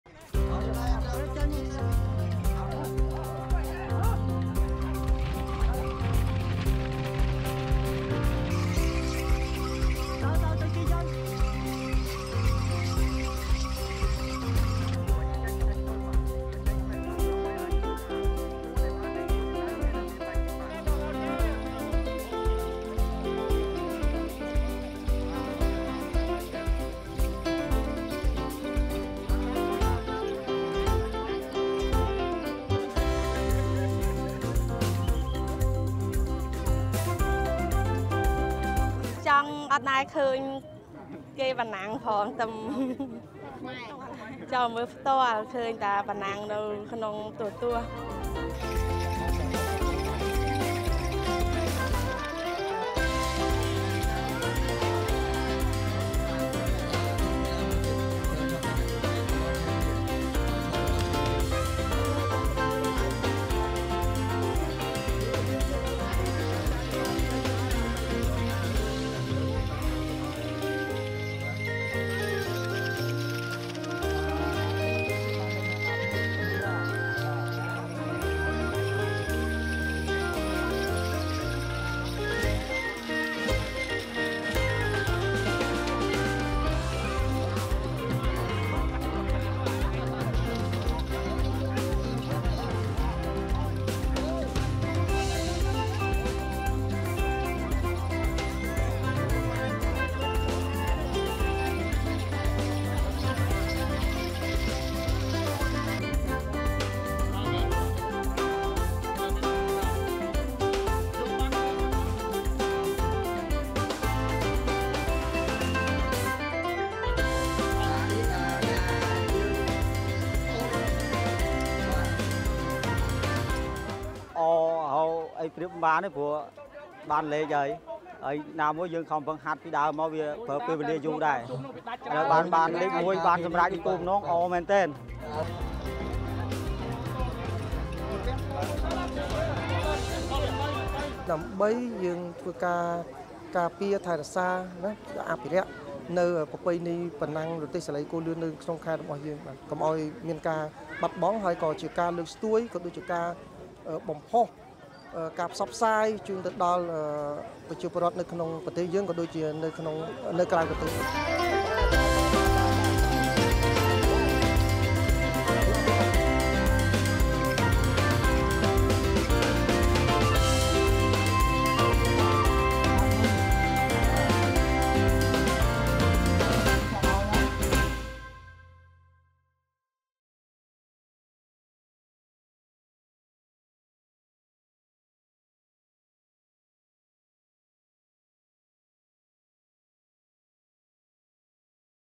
LOOK: Cambodia's annual ox cart race enthralls crowds | ANC
On The Scene: Cambodian farmers and their speedy oxen from across the country took part in an annual ox cart race on Sunday (April 7), heralding the Khmer New Year and the rice-harvesting season which kicks off mid-April